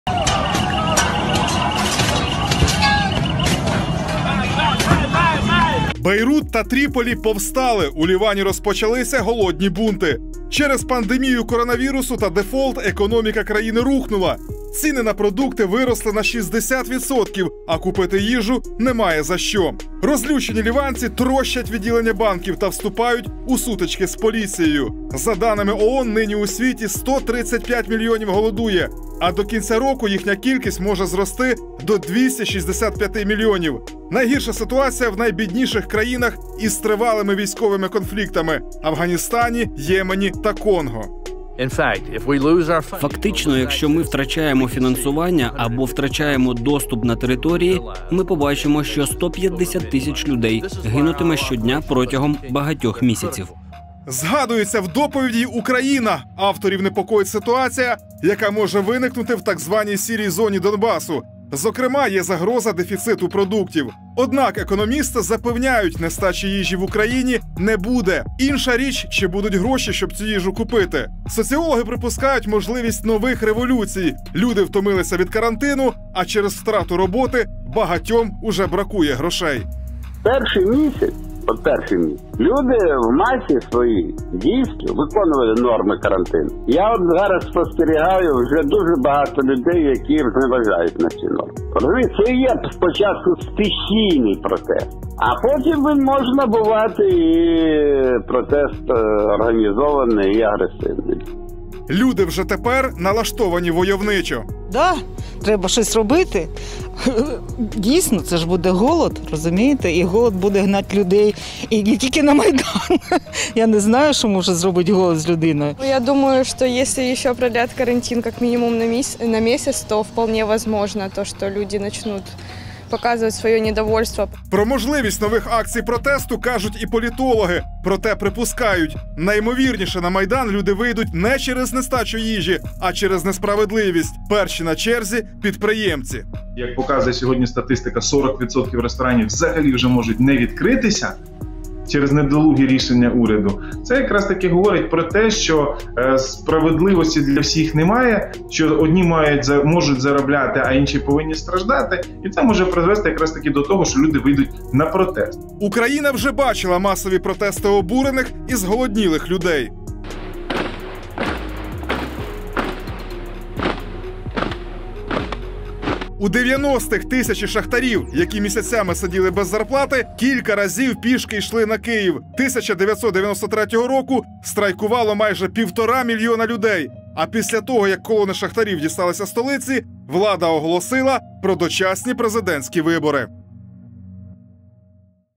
ООН прогнозує глобальну нестачу їжі
Світовий голод біблійних масштабів. Організація об’єднаних націй пророкує - не встигне людство оговтатися від пандемії КОВІД-19, як перед планетою постане новий виклик - масштабна нестача їжі. Днями всесвітня продовольча програма, одна з фундацій організації об’єднаних націй оприлюднила прогноз на 2020 рік. Його автори припускають - до кінця року кількість людей, які потерпають від нестачі їжі зросте на 130 мільйонів. Постраждають мешканці щонайменше 55 країн. У дослідженні йдеться і про Україну. Чи очікують нас голодні бунти - Павло Васильєв дізнавався.

4 канал – унікальний інформаційний суспільно-політичний канал. У нашому живому етері лише гарячі диспути та потрібна інформація.